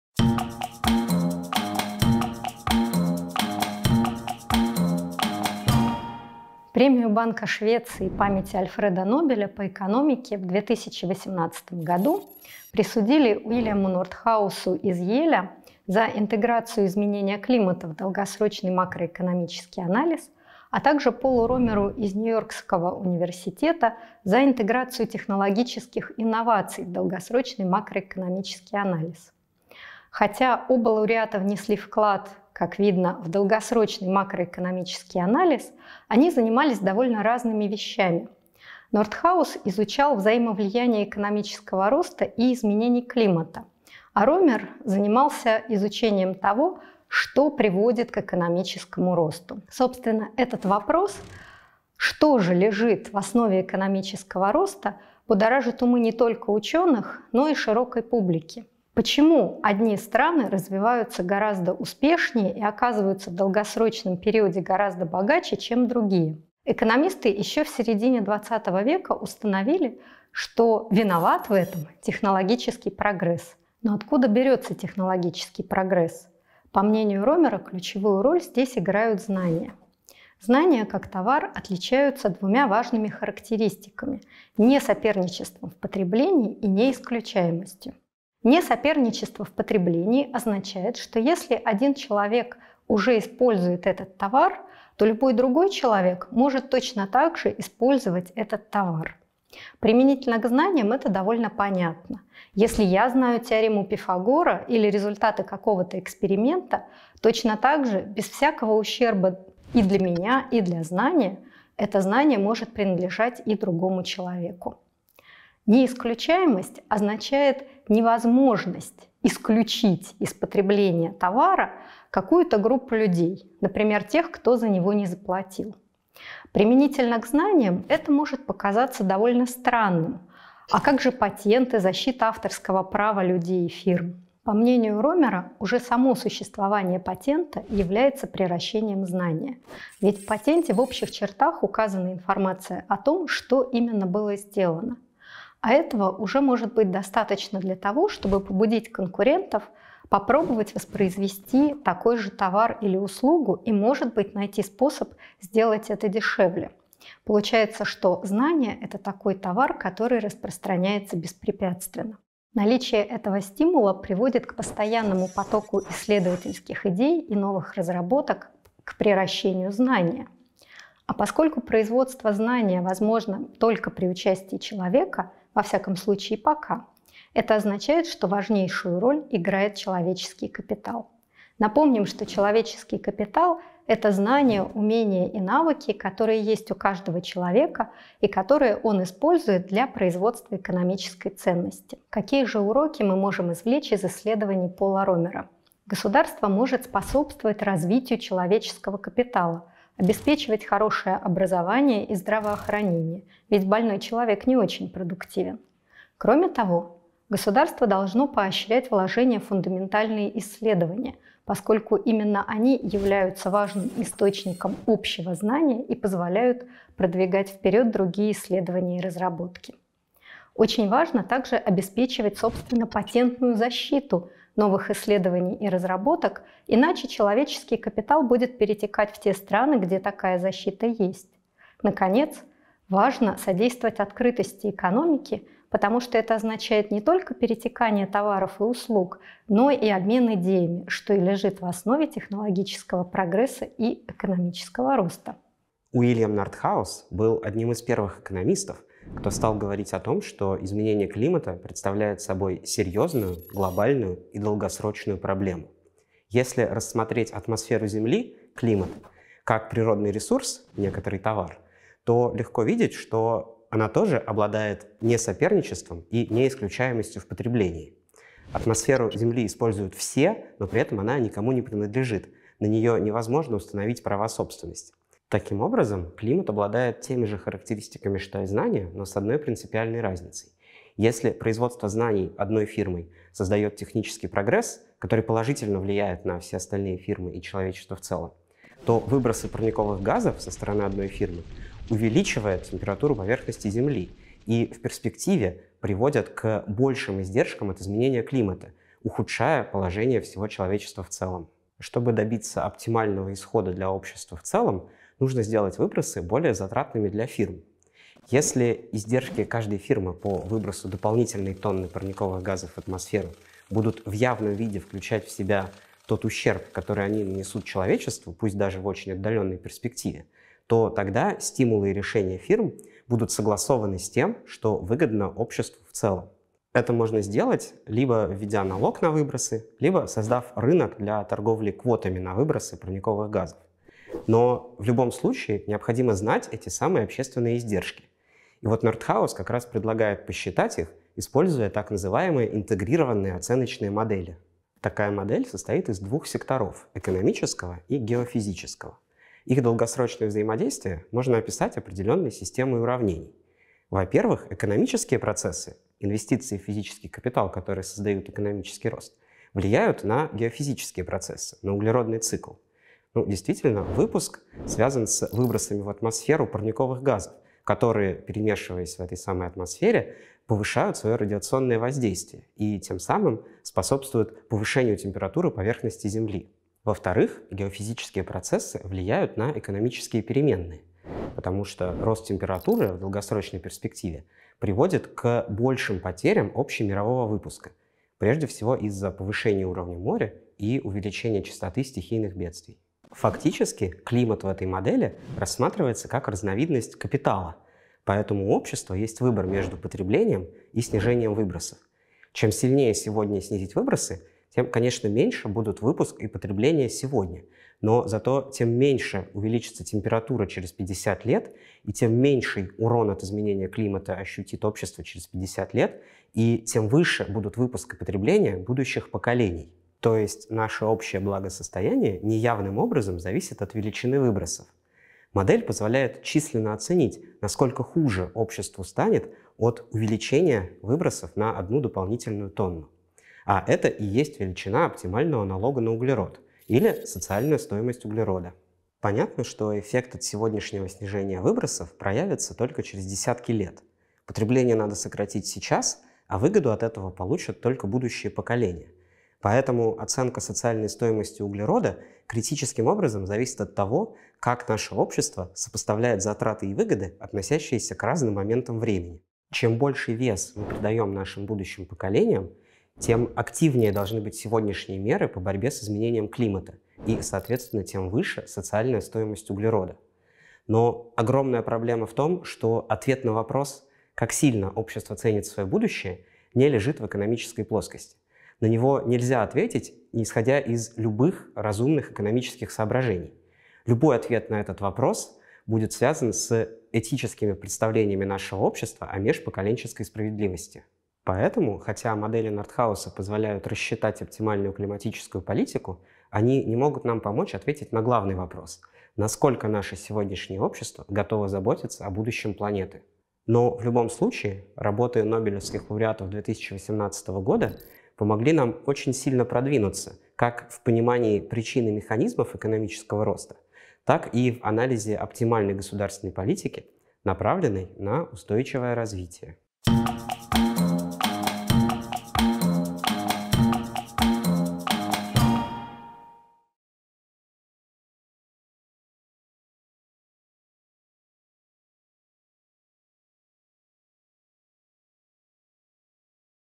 Нобелевская премия по экономике-2018: как обеспечить долгосрочный экономический рост.
Вместе с экономистами Европейского университета продолжаем простым языком рассказывать о лауреатах премии и значимости их достижений.
Нобелевскую премию по экономике в 2018 году присудили Полу Ромеру из Нью-Йоркского университета за исследования технического прогресса как основной причины долгосрочного экономического роста, и Уильяму Нордхаусу из Йельского университета за изучение взаимосвязи изменения климата и экономического роста. Работы лауреатов 2018 года не только помогли нам в понимании фундаментальных причин экономического роста, но и показали, какой должна быть оптимальная государственная политика, направленная на устойчивое развитие. Если вам интересно узнать больше — смотрите наш ролик!

Продолжить дискуссию о нобелевских лауреатах или задать вопросы про обучение на факультете экономики можно в любом из удобных для вас каналов: